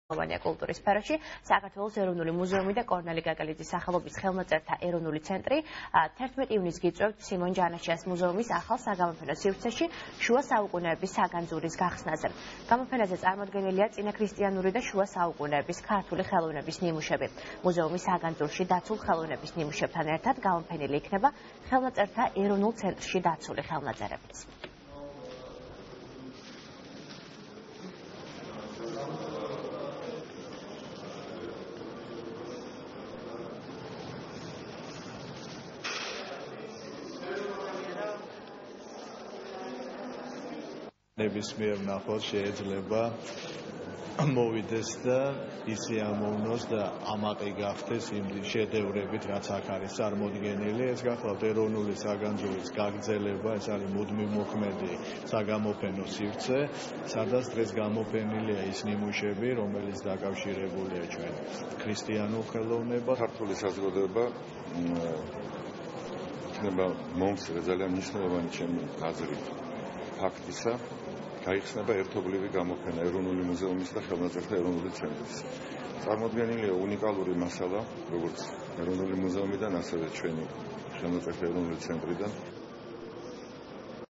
გამოფენა ,,შუა საუკუნეების საგანძური"
11 ივნისიდან საქართველოს ეროვნული მუზეუმი და კორნელი კეკელიძის სახელობის ხელნაწერთა ეროვნული ცენტრი გიწვევთ სიმონ ჯანაშიას სახელობის საქართველოს მუზეუმის ახალ საგამოფენო სივრცეში გამოფენაზე ,,შუა საუკუნეების საგანძური".